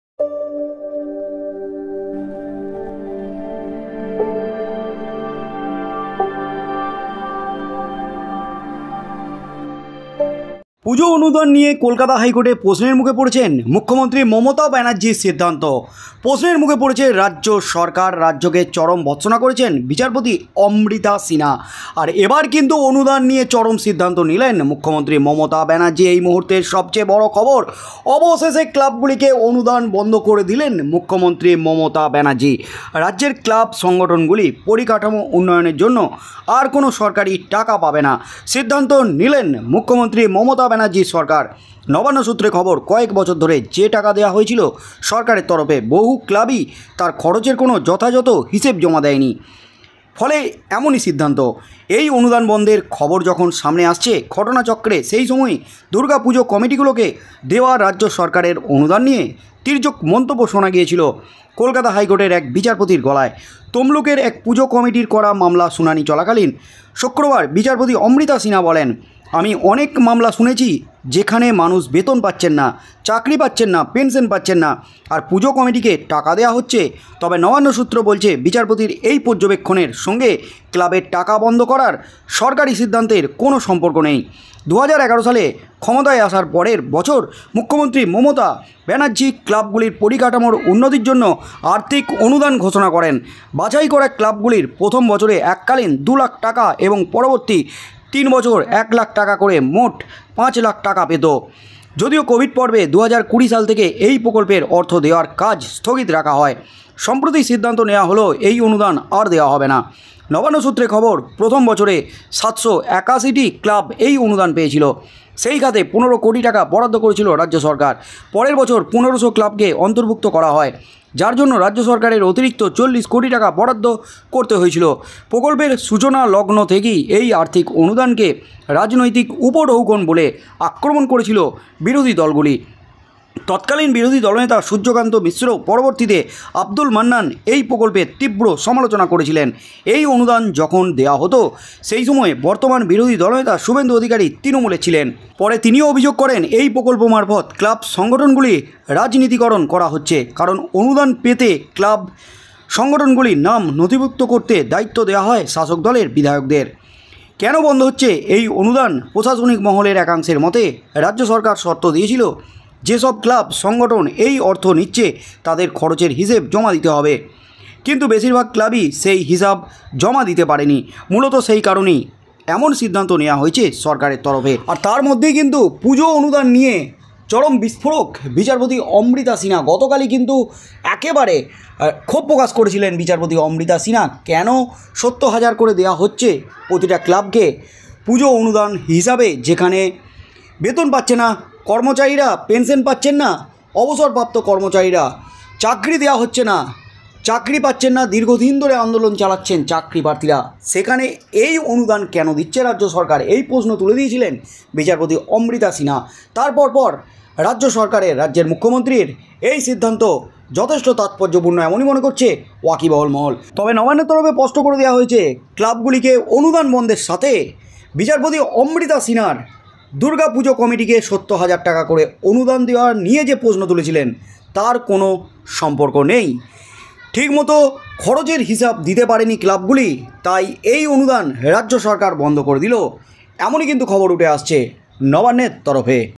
পুজো অনুদান নিয়ে রাজ্যকে হাইকোর্টের কড়া হুঁশিয়ারির পরই অনুদান বন্ধের বেনজির সিদ্ধান্ত মমতার!
ধন্যবাদ

Keyword⏩